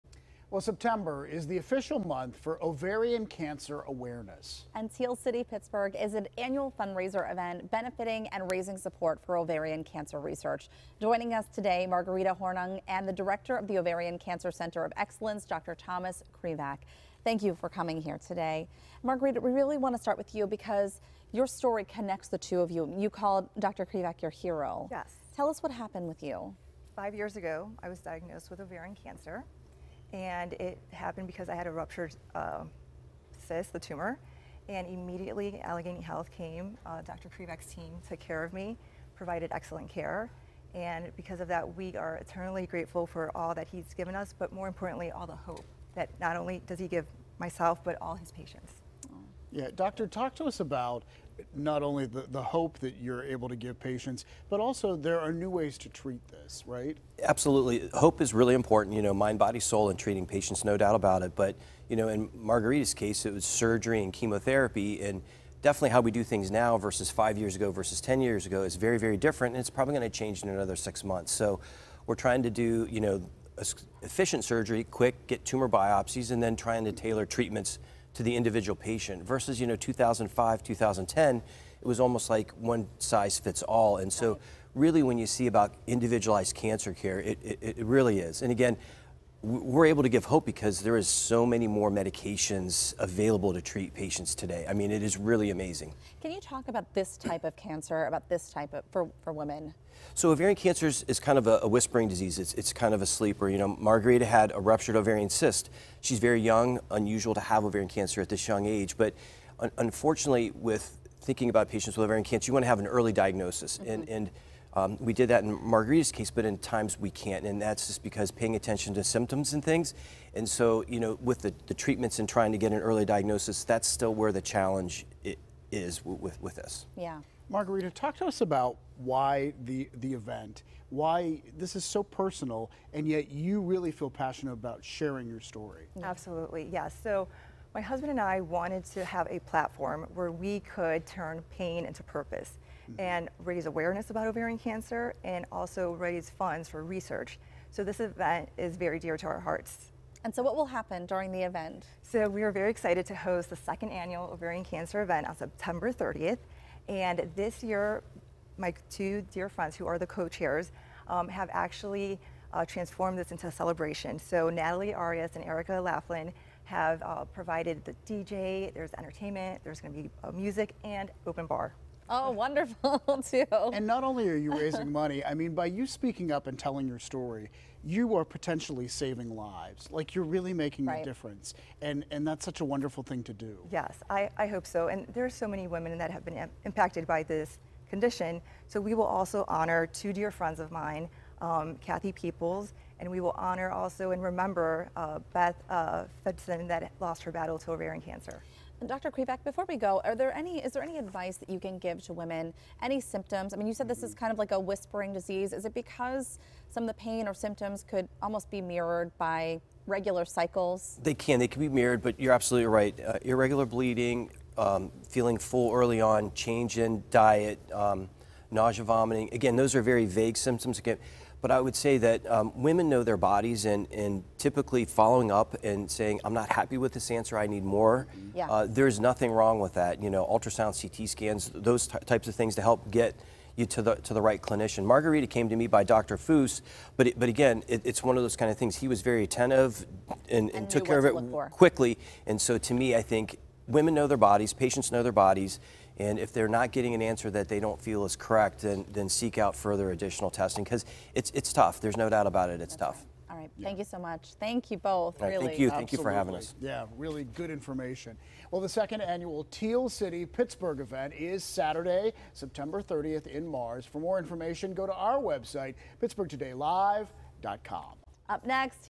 Learning more about Teal City Pittsburgh, benefitting ovarian cancer research
September is the official month for ovarian cancer awareness and Teal City Pittsburgh is an annual fundraiser event benefitting and raising support for ovarian cancer research. We were joined by the director of the Ovarian Cancer Center of Excellence, Dr. Thomas Krivak and Margarita Hornung.